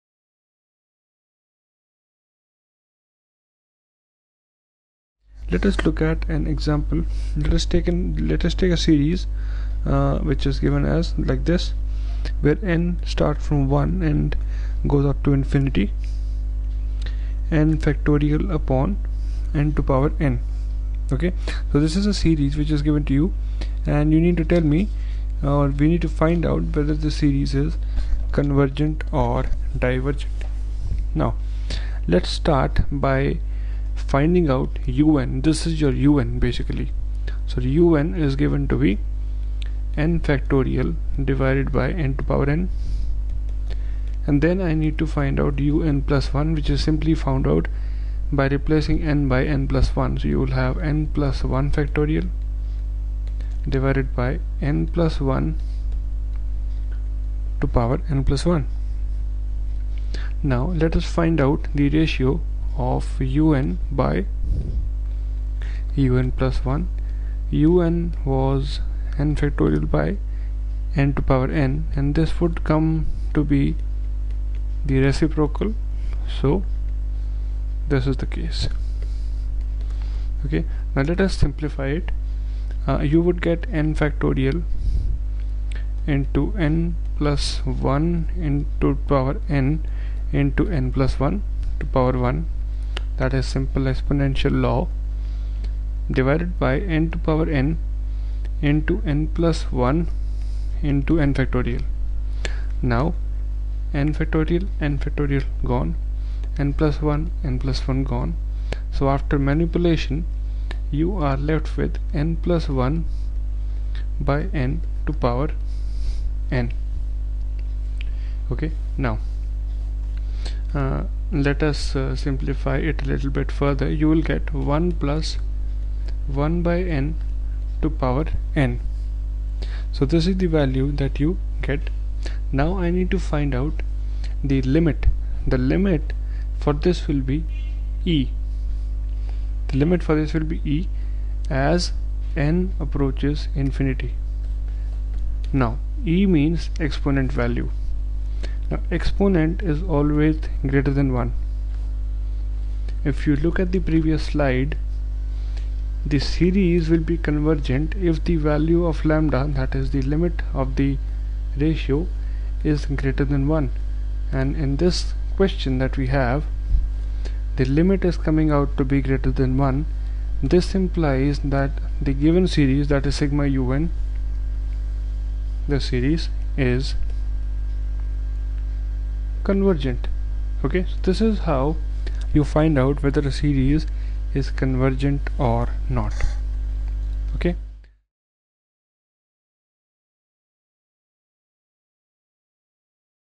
Alembert's Ratio Test Examples - Engineering Mathematics & General Aptitude - GATE Mechanical
This video covers Alembert's Ratio Test Examples. Learn how to find whether the series is convergent. This is a sub-topic under the Engineering Mathematics & General Aptitude in GATE Mechanical exam.